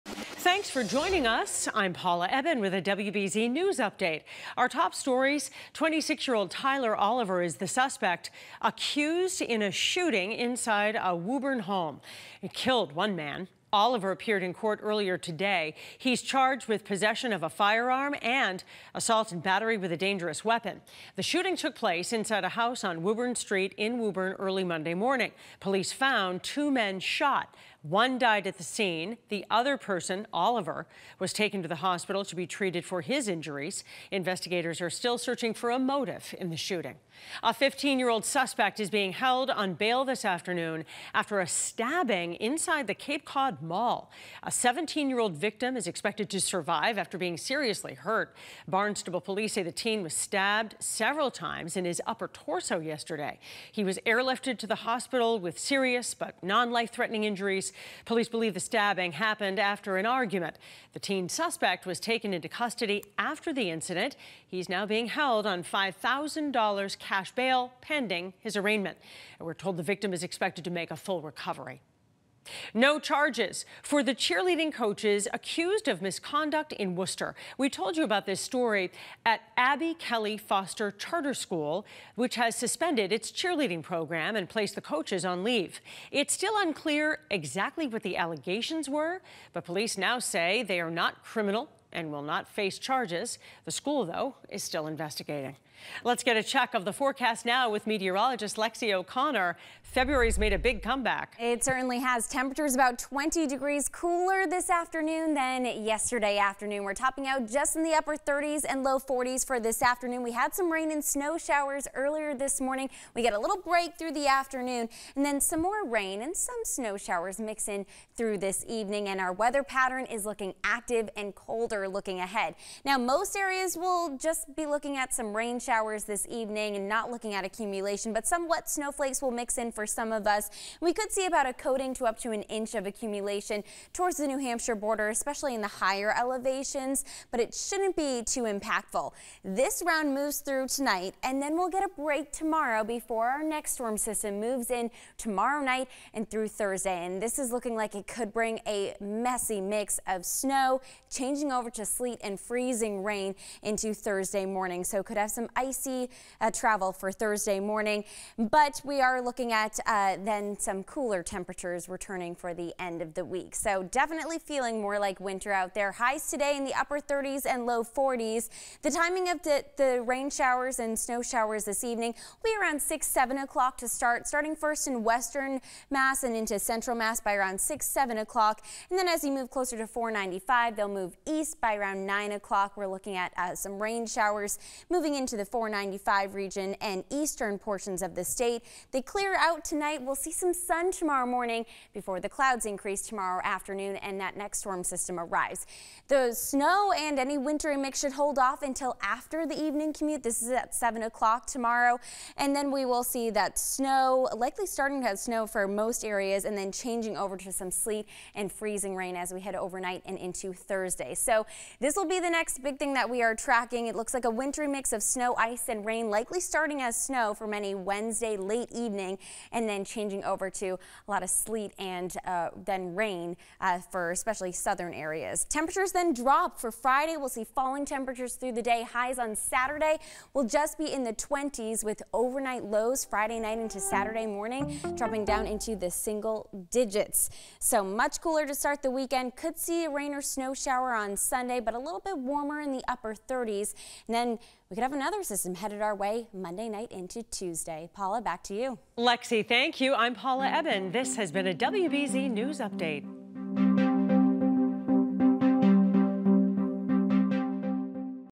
WBZ News update for February 21
Suspect arrested in Woburn shooting; 15-year-old taken into custody for Cape Cod stabbing; Worcester cheerleading coaches not charged; Next weather update.